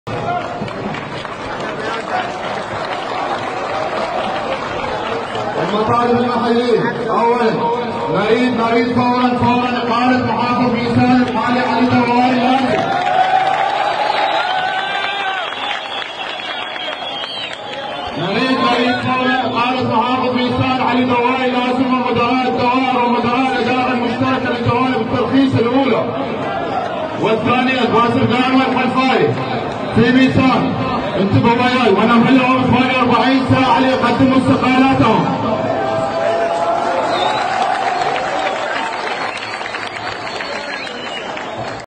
متظاهرو ميسان يطالبون بإقالة الحكومة المحلية في المحافظة
ــــــــــــــــــــــــــــــــ